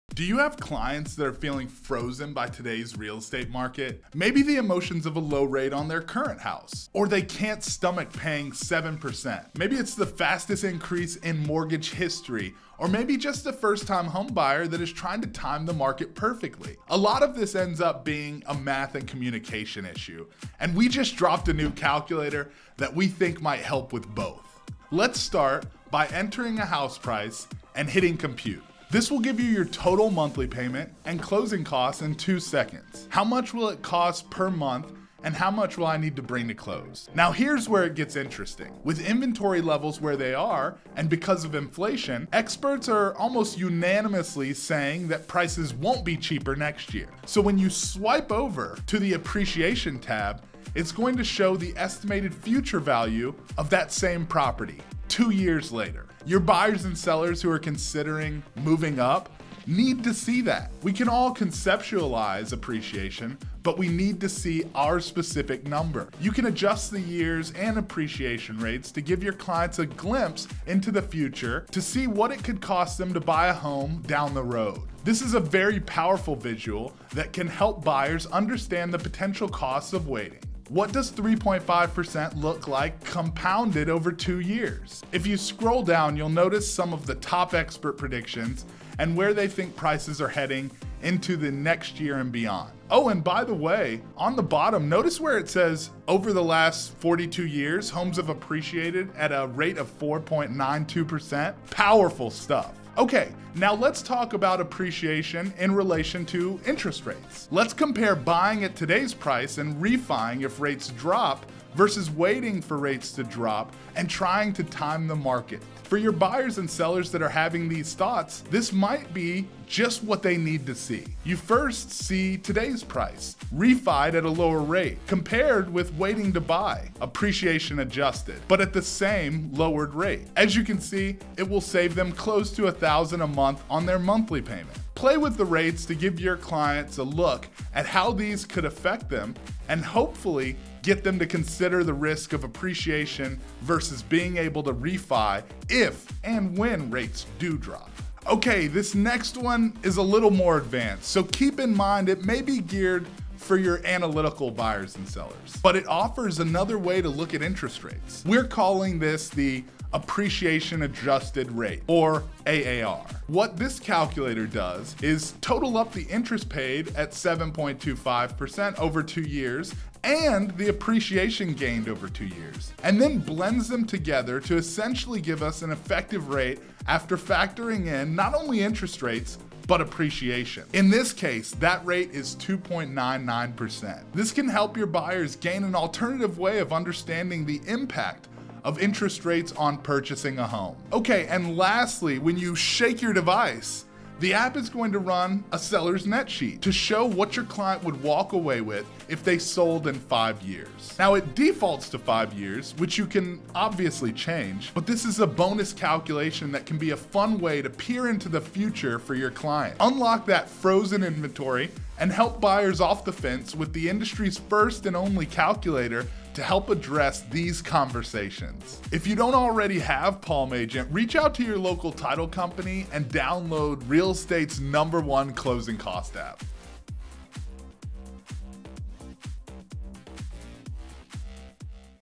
Buy Now or Buy Later Calculator Tutorial
Learn how to use our NEW calculator to handle conversations with anyone who is thinking about waiting to buy in hopes of securing a lower rate!